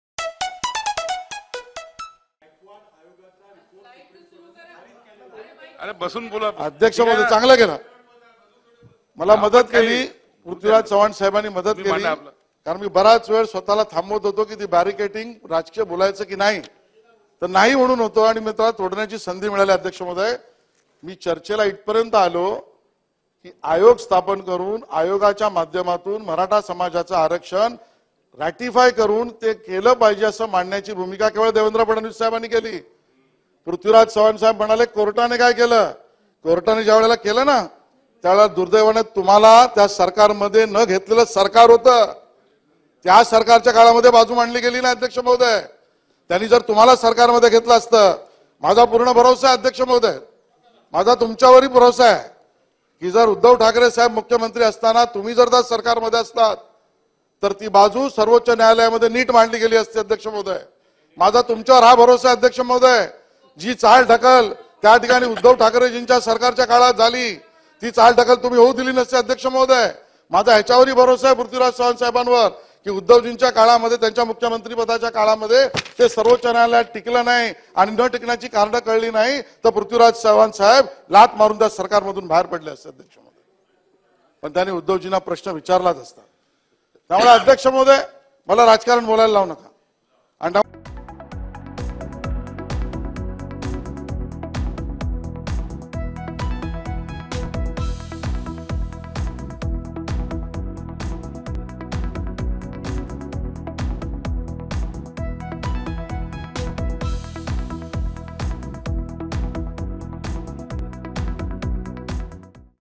Ashish Shelar पृथ्वीराज चव्हाण यांना मराठा आरक्षण आणि महाविकास आघाडी सरकारवरून काय म्हणाले?
आशिष शेलार पृथ्वीराज चव्हाण यांना मराठा आरक्षणावरून काय म्हणाले?